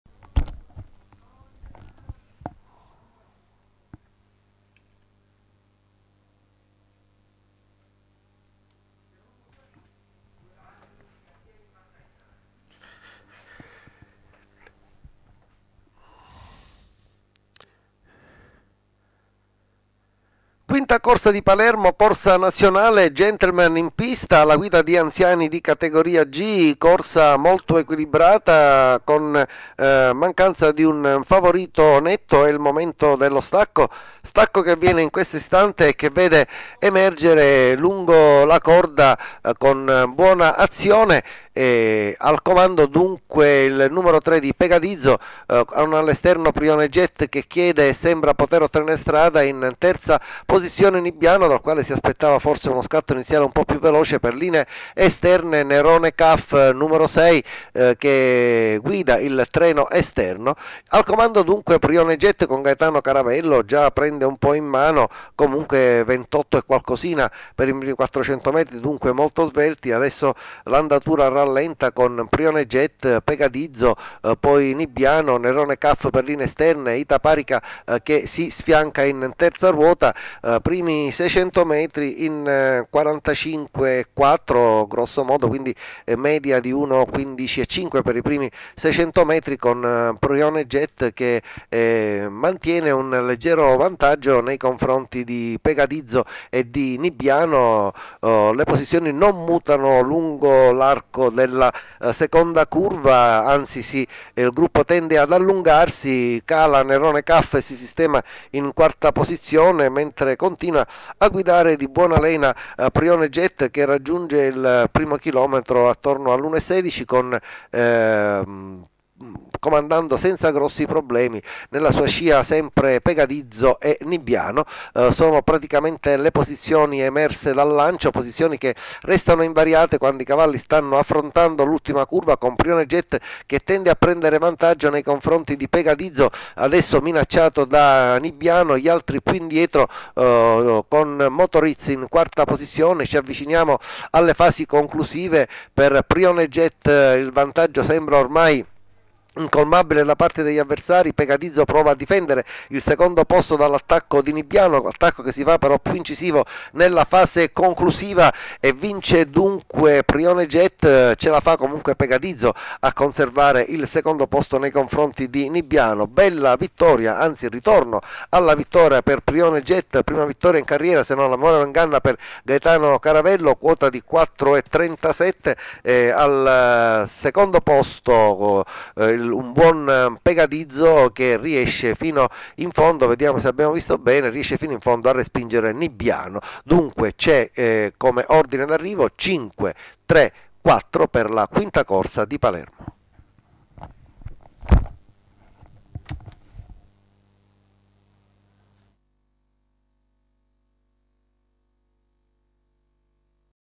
IPPODROMO PALERMO "LA FAVORITA"-- 5° CORSA
5° CORSA Premio KHARKIV Mt.1620 (Cat.G - Gentl.- Ris.)
Cronaca: Pietro Di Salvo